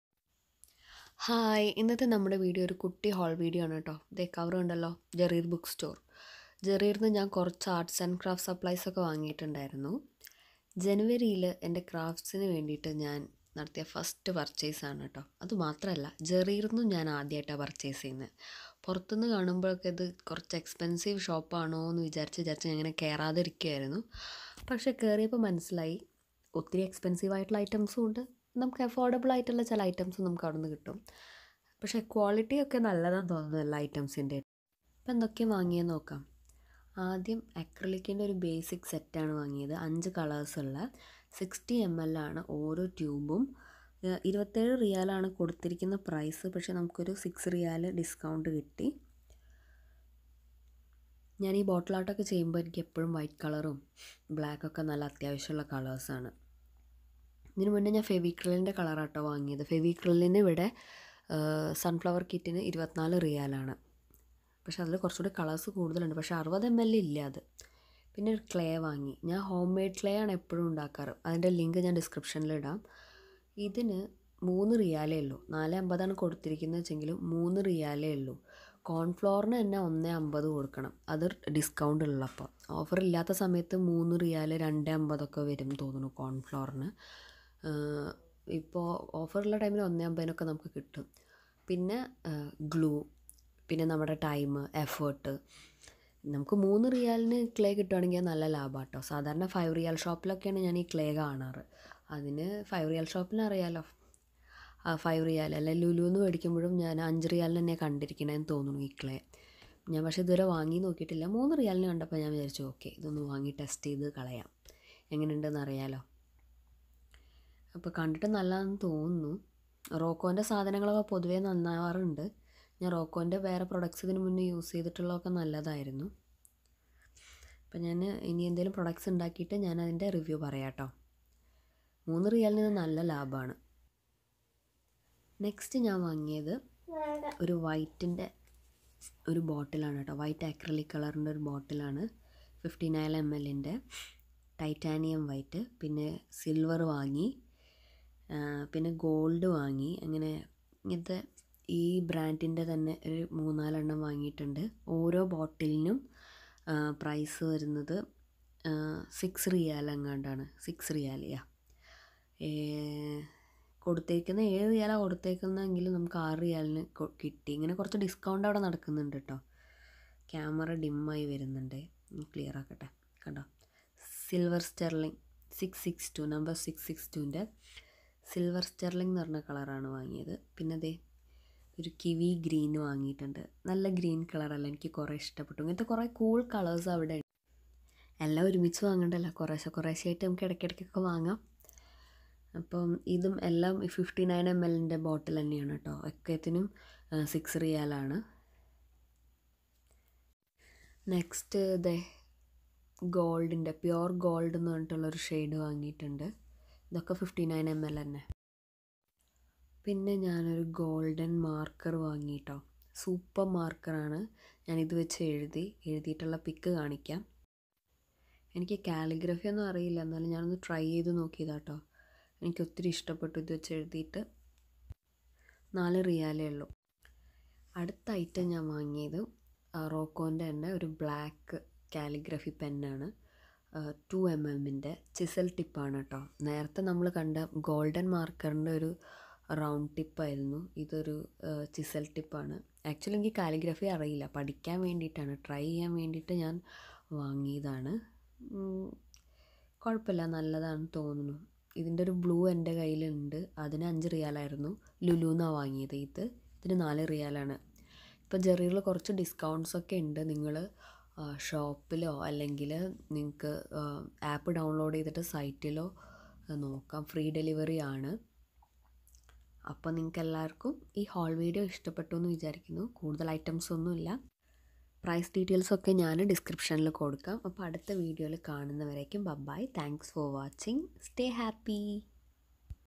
Arts and crafts supplies jarir book store KSA
1.MBGI acrylic colour set - SR 21

2.Roco air-dry modelling clay  - SR 3

3.Marker paint gold- SR 4

4.paint acrylic titanium white, kiwi green- SR 6 each

5.paint acrylic pure gold, sterling silver - SR 7

I bought these items from Dammam Jarir bookstore on 19/1/2020

prices and discounts may vary.

homemade clay video

craft haul
arts and craft. supplies